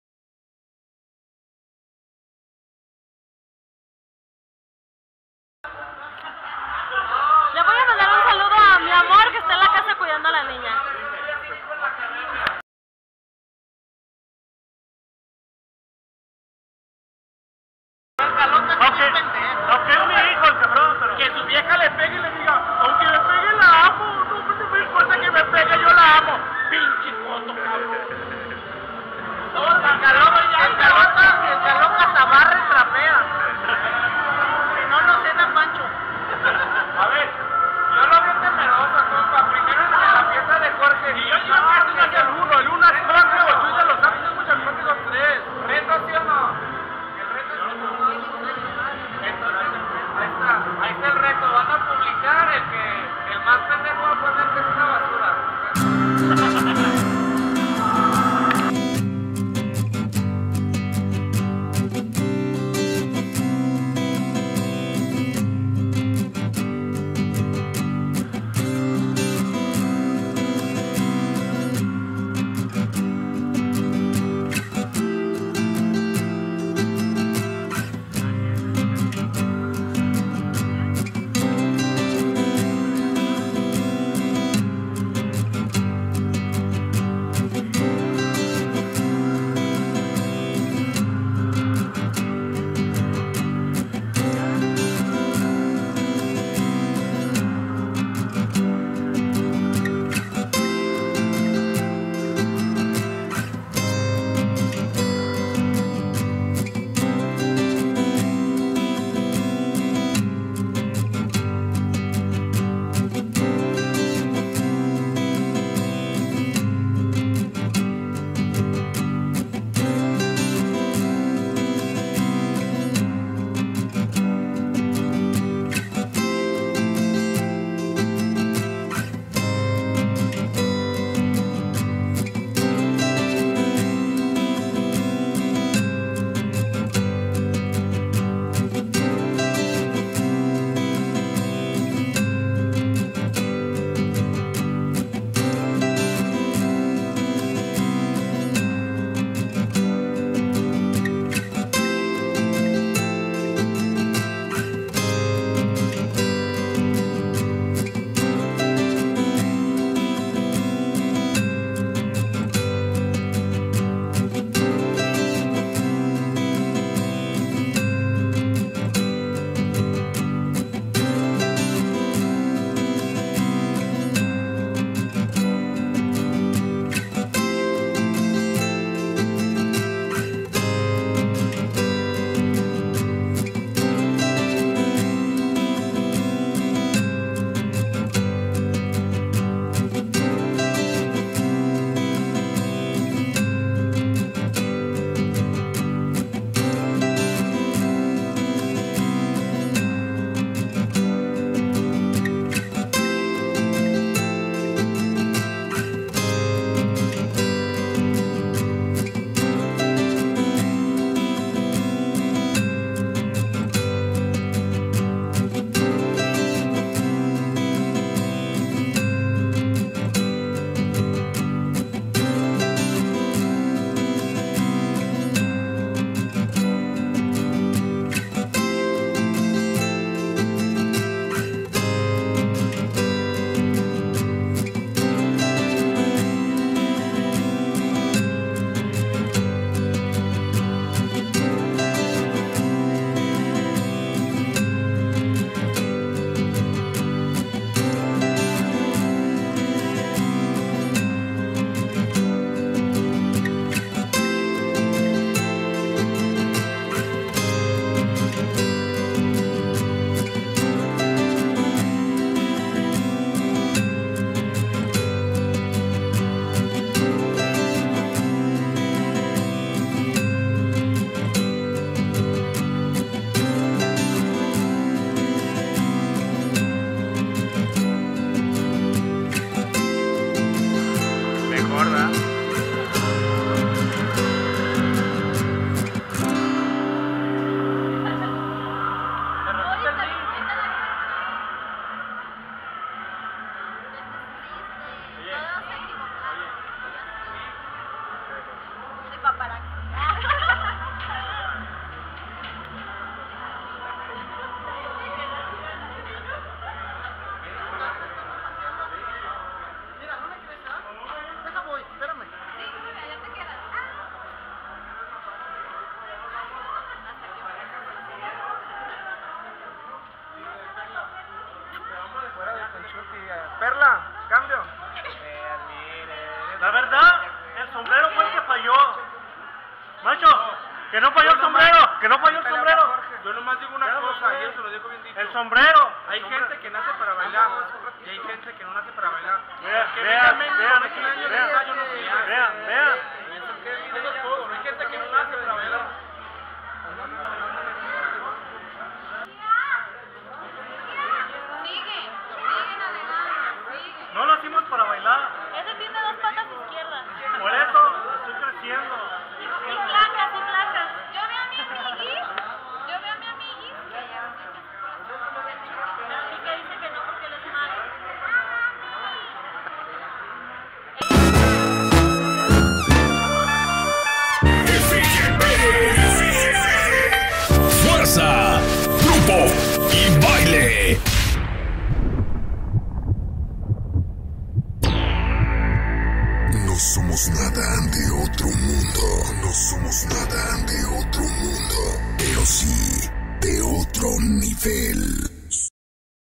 27 expotequila EN TLAQUEPAQUE 06 DE SEPTIEMBRE 2014
UN POQUITO DE LO QUE PASO EN EXPOTEQUILA EN EL CENTRO CULTURAL DE EL REFUGIO ESTE SABADO
GRACIAS A TODOS LOS ORGANIZADORES